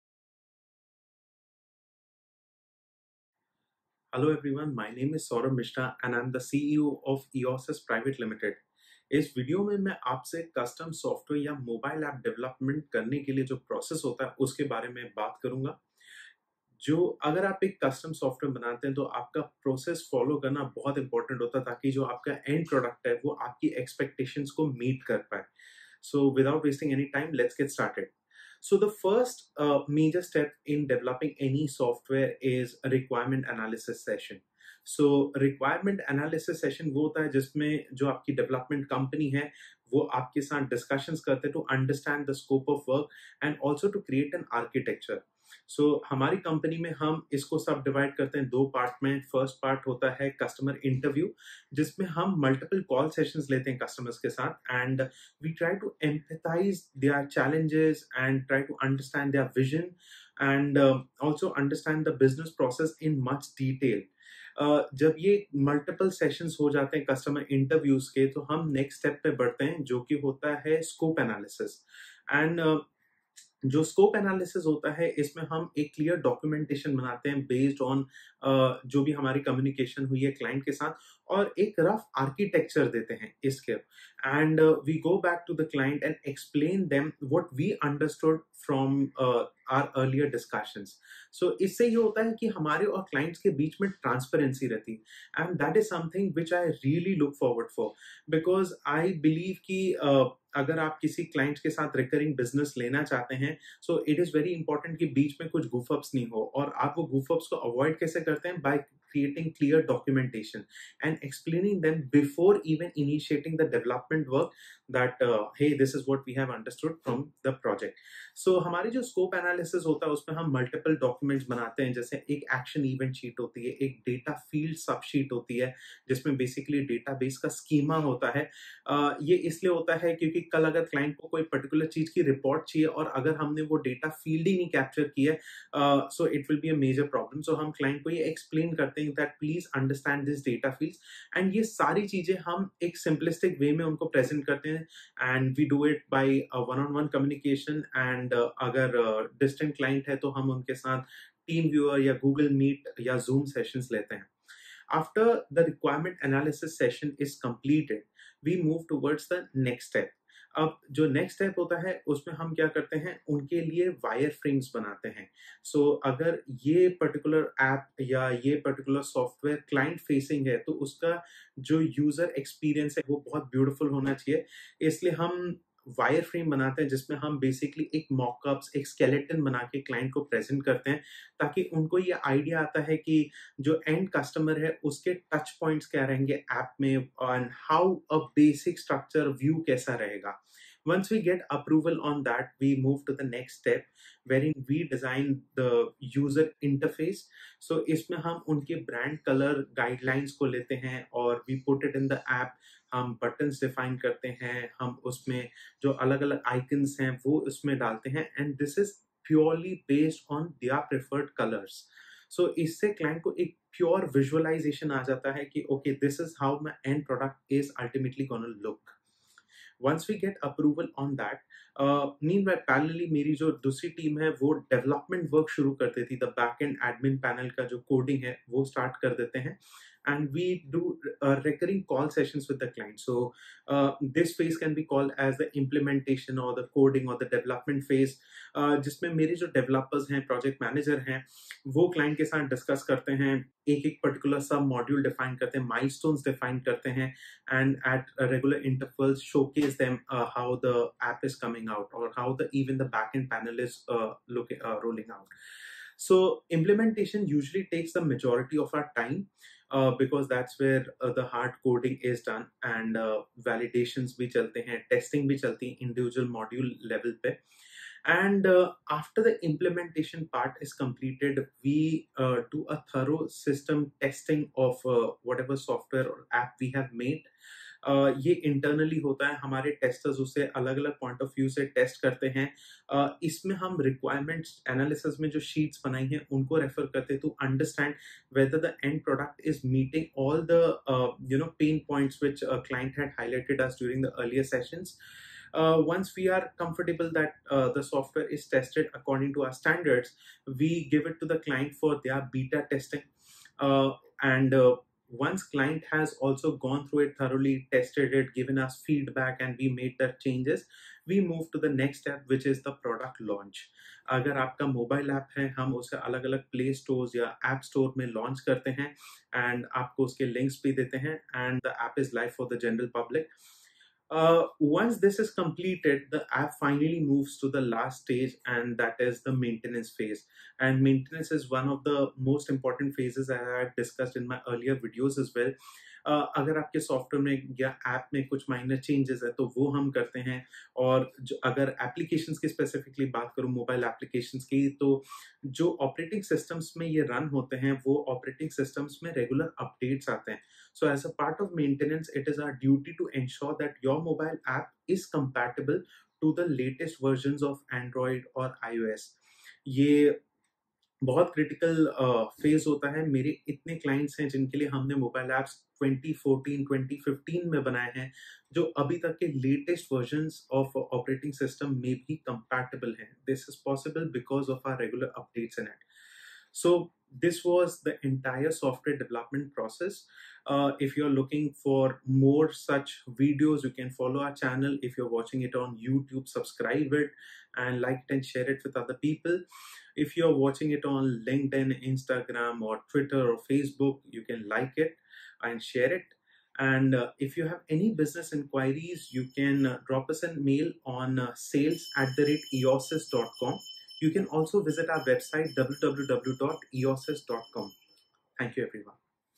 App Development Process | Custom Software Development Process
In this video, we have explained the app development process that we follow to build custom software and apps.

If you want to read it in greater detail, you can

The steps are as follows:

1. Requirement Analysis
This is one of the most critical steps of the software development process. The success of your app development depends on how well the development team understood your requirements.

In my company, we have created two sub-steps within this step.

1.1. Client Interview
In this step, we conduct multiple meetings/call sessions with the clients to understand the business flow and their expectations from the app.

1.2. Scope Analysis
In this step, we translate the requirements into technical documents. We prepare an action-event sheet, data fields sheet, special cases, and so on. In the action-event sheet, we list down the actions that need to be triggered on the occurrence of a specific event.

These documents are shared with the client. We explain to them the significance of these documents. This step helps us establish greater transparency with our clients.

2. Wireframes and User Experience
In this step, we create wireframes of the mobile app or web app. This step helps us clearly define the user experience of the app.

3. User Interface
In this step, we create the actual screens of the app. We use the brand guidelines shared with us by the client. After this step of the software development process, clients get an actual idea of the look of the final app.

4. Implementation
In this step, our team starts coding the software or app. The development of the backend admin panel begins immediately after the requirement analysis session.

This step consumes most of the time in the app development process. We conduct regular sessions with the client to showcase them the progress of their app development. We also test the software at a modular level to check its functionality.

5. Testing
We do thorough testing of the app internally. In the case of mobile apps, we test it on different versions of the android and iOS to check its compatibility with them.
Once the app has passed our internal standards of testing, we share it with the clients for final approval

6. Beta Testing and Optimisation
In this step, the client rolls out the app internally to get feedback from their team members. We perform the changes suggested by the client. If it's approved, we move to the next step

7. Product Launch
The app is launched on Google Play Store and Apple App Store and available for the public. In the case of the web app, it is made live on their servers

8. Maintenance
In this step, we regularly update the apps to ensure that they are compatible with the latest versions of the android and iOS. We also perform bug fixing and implementing changes for the client

If you are looking to build a mobile app check out